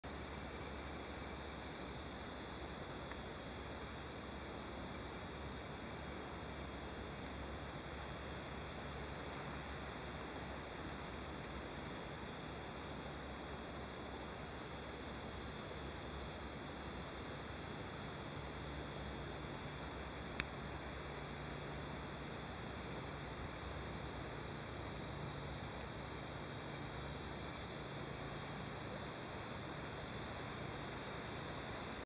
Turtle Family at the Hillsborough River State Park
Big Turtle Family sunbathing in the middle of the beautiful Hillsborough River.Recorded August 2011